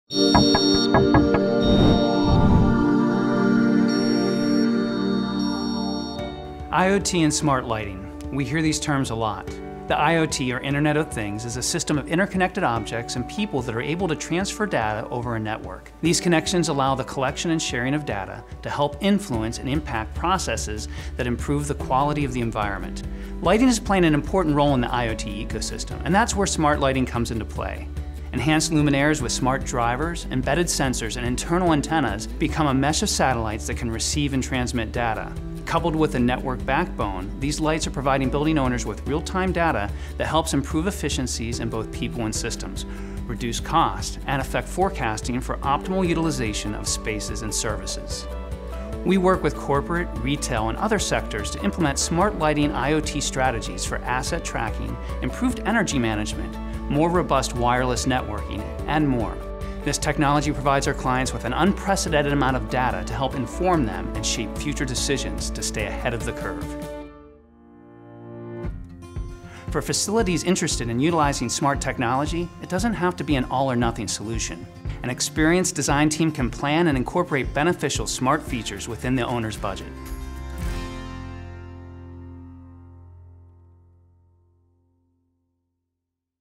IoT and Smart Lighting Explained
Jered Widmer, IALD, MIES, explains the important role lighting and The Lighting Practice is playing in the IoT ecosystem. He also provides tips for facilities interested in utilizing smart technology.

Video filmed and produced by Oak Leaf Media